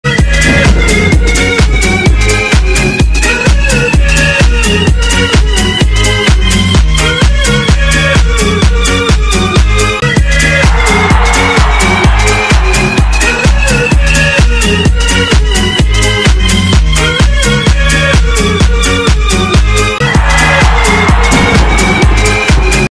Who is Daichi Kamada 鎌田 大地 football player club Eintracht Frankfurt. FOCUS FOCUS GOAL!!!!
This guy works hard and gets the SCORE!!!
FRANKFURT to SHINE!